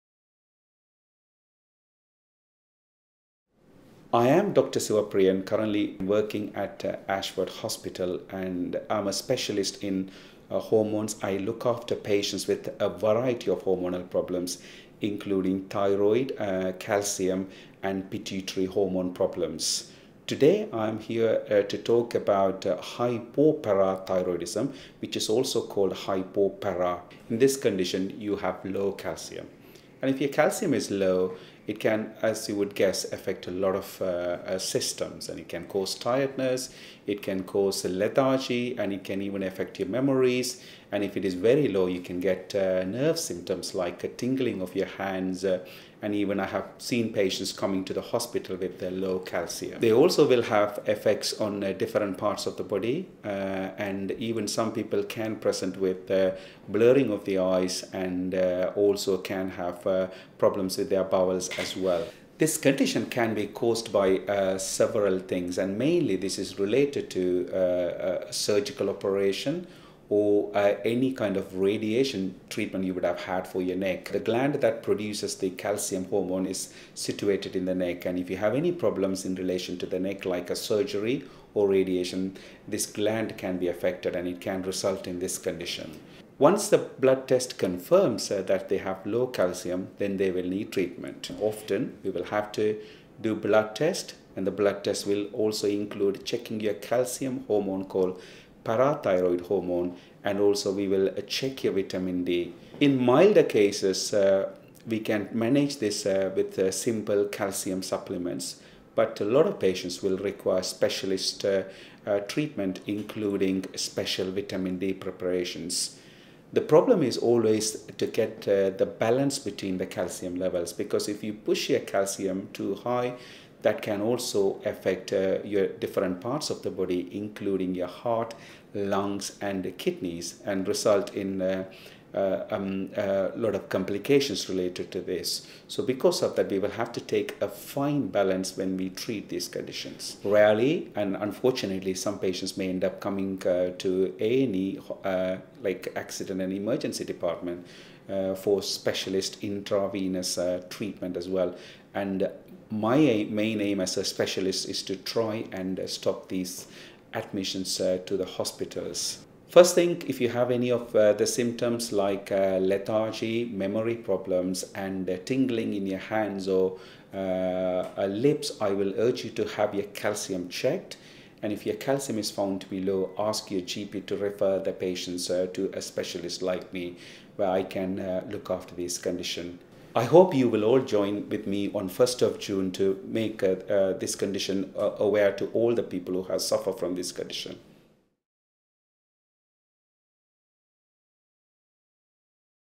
Dr Sivappriyan, Consultant Physician in Diabetes and Endocrinology
Dr Sivappriyan, Consultant Physician in Diabetes and Endocrinology explains the symptoms and treatments for hypoparathyroidism.